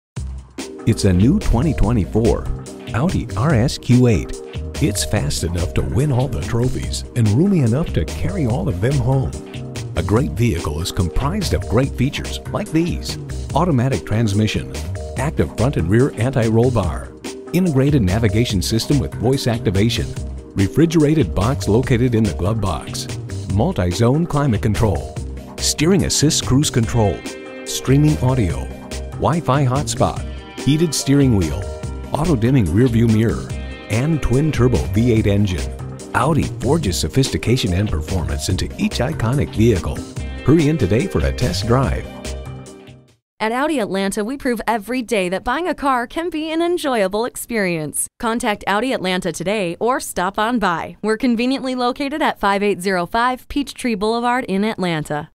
New 2024 Audi RS Q8 Atlanta Alpharetta, GA A24907 - SOLD
Year: 2024
Make: Audi
Model: RS Q8
Trim: 4.0T
Engine: 4.0L V8 Turbocharged DOHC 32V LEV3-ULEV125 590hp
Transmission: 8-Speed Automatic with Tiptronic
Color: Gray
Interior: BLACK
Stock #: A24907
VIN: WU1ARBF10RD000174

Address:
5805 Peachtree Boulevard